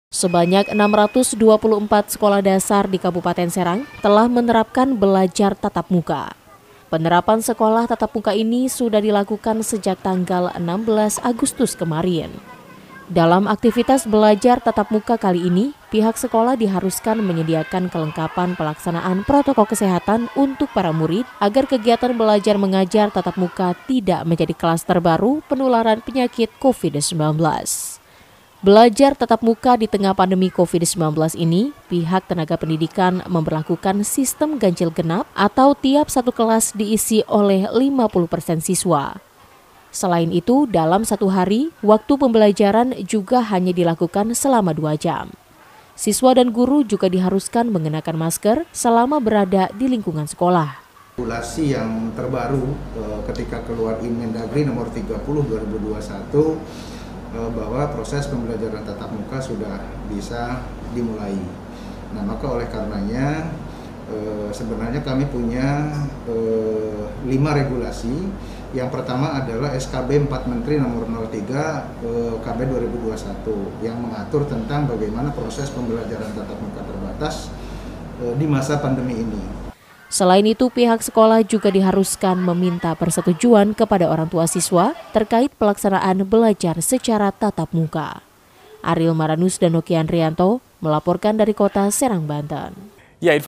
RATUSAN SEKOLAH DASAR DI KABUPATEN SERANG TELAH MELAKSANAKAN KBM SECARA TATAP MUKA
RATUSAN SEKOLAH DI KABUPATEN SERANG, PROVINSI BANTEN TELAH MENERAPKAN BELAJAR TATAP MUKA. DALAM AKTIVITAS BELAJAR TATAP MUKA KALI INI, PIHAK TENAGA PENDIDIKAN DIHARUSKAN MENYEDIAKAN KELENGKAPAN PROTOKOL KESEHATAN UNTUK PARA MURID,bUNTUK MENGHINDARI KLASTER PENYEBARAN COVID-19.